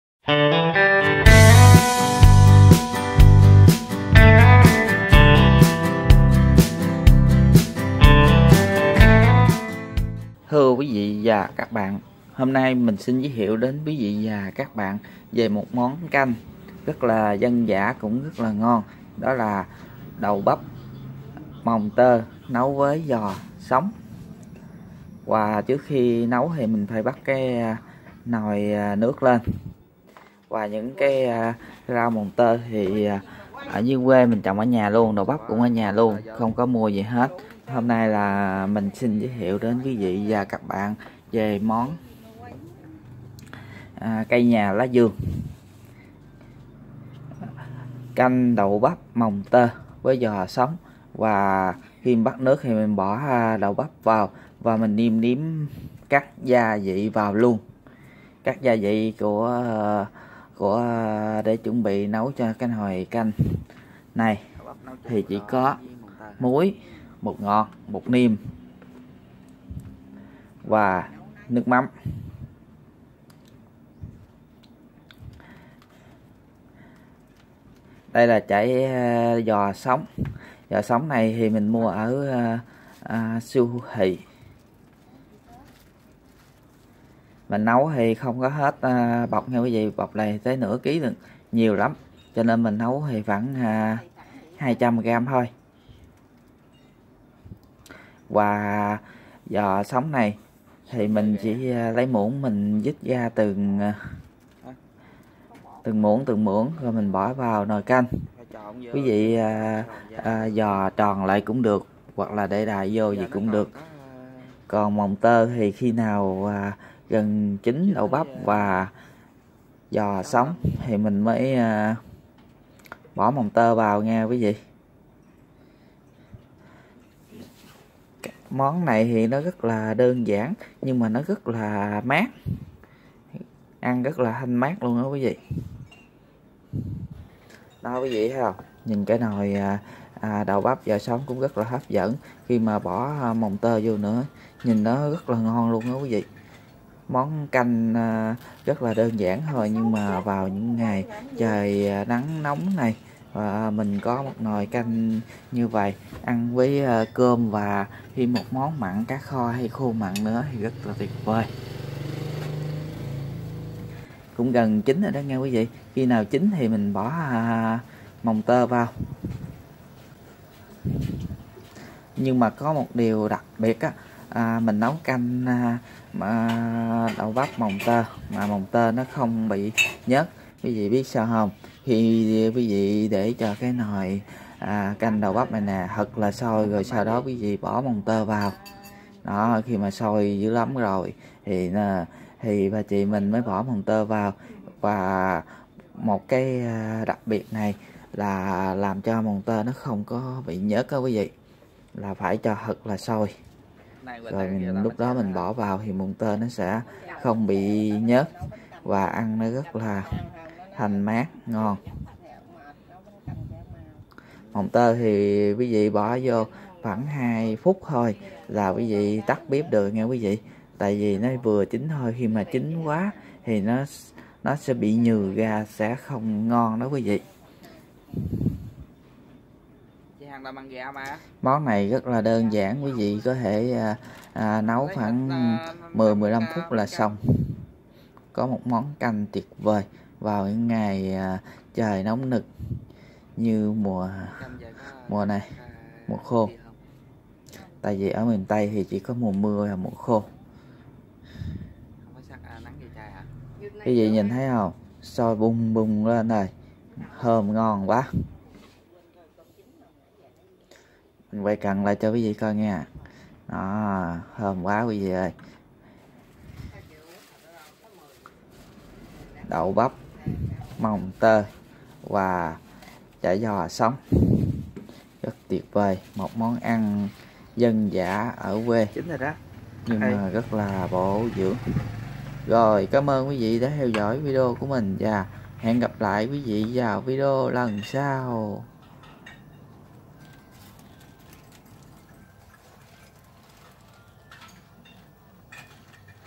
NẤU CANH ĐẬU BẮP MỒNG TƠI VỚI GIÒ SỐNG - món ngon dễ làm
nấu canh đậu bắp mồng tơi với giò sống một món ăn rât dân dã rât bổ dưỡng ở vùng quê,cây nhà lá vườn, nấu củng rất đơn giản không mất thời gian nhiều, canh đậu bắp mồng tơi giò sống rât thanh mát đậm đà hương vị,